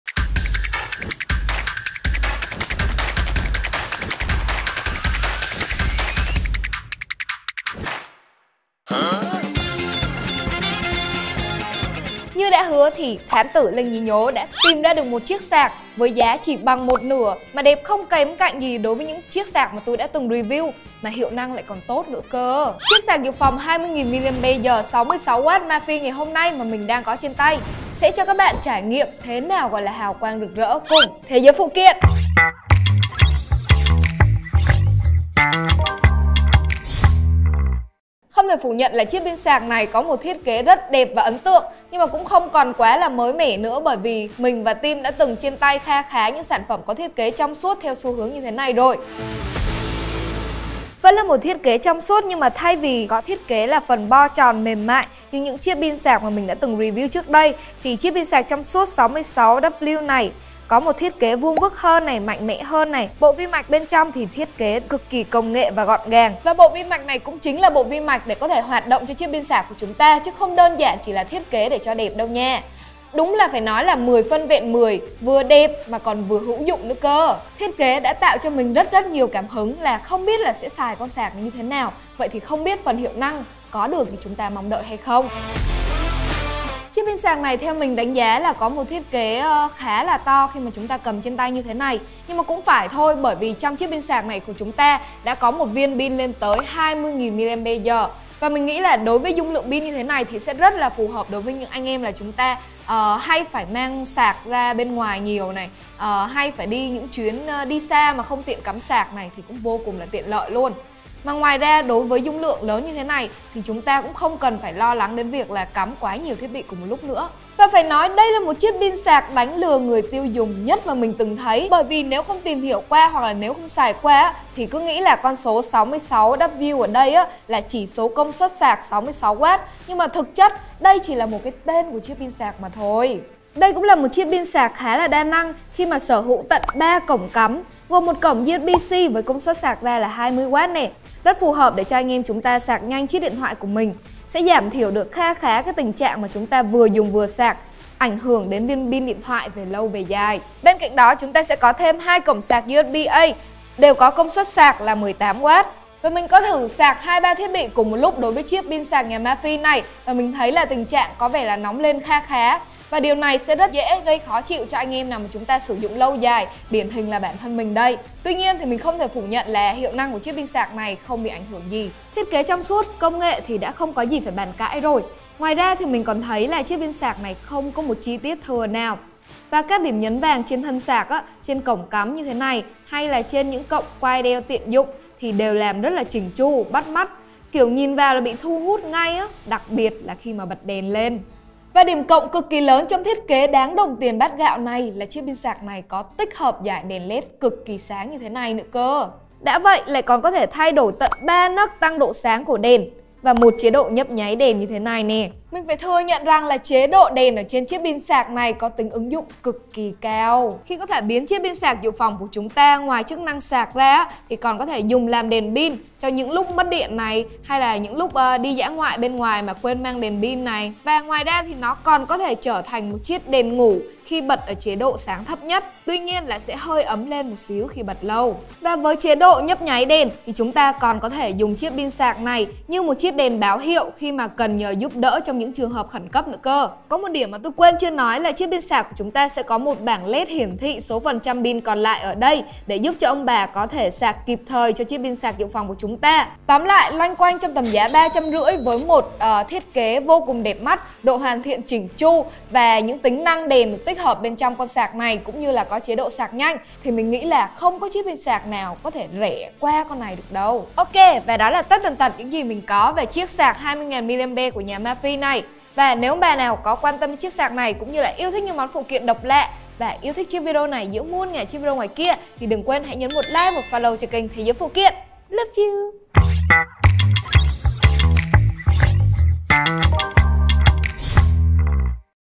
Chiếc sạc MAPHIE 66W giá chưa đến 500K sẽ cho bạn cảm nhận "hào quang rực rỡ"
Sạc dự phòng WIWU 20.000mAh 66W với mức gáic chưa đến 500k nhưng hiệu năng lại cực kì đáng mong đợi. Chiếc sạc dự phòng nhà WIWU này còn là một chiếc sạc dự phòng đa năng khi có tích hợp thêm dải đèn LED cực kì bắt mắt cùng nhiều chế độ.
Đừng quên Sub kênh để có nhiều video hấp dẫn nhé!